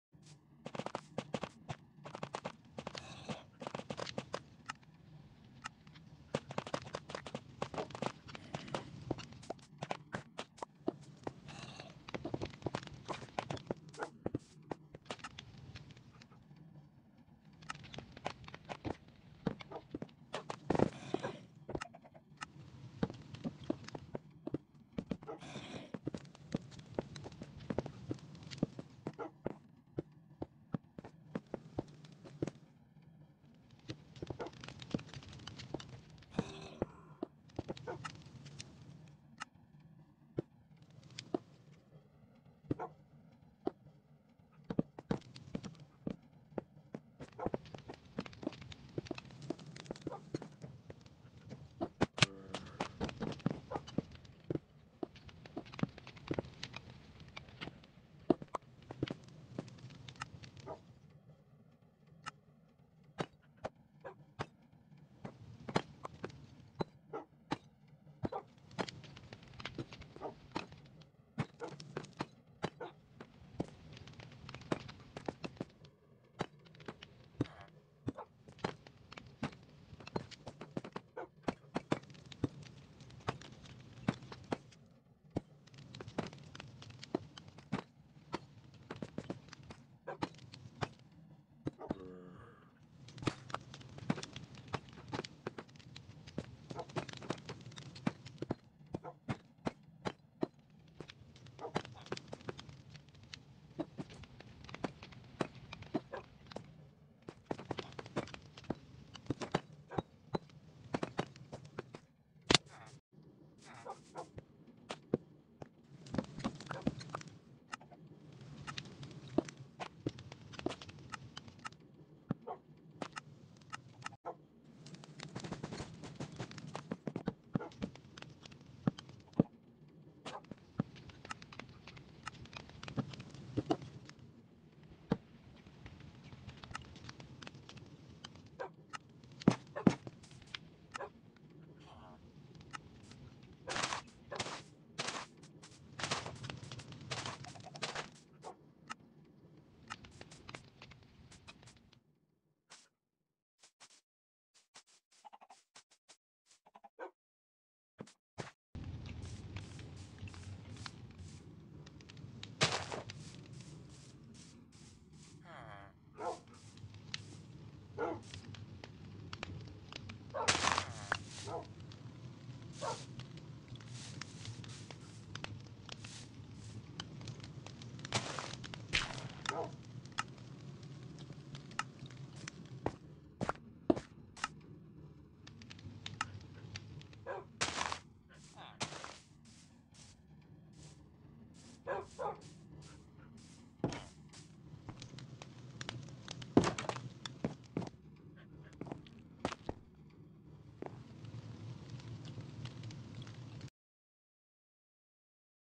This WORKSHOP I built in my backyard on my SMP holds a dark secret...
I built a small workshop in my backyard on my SMP. However, it holds a dark secret! I used Birch!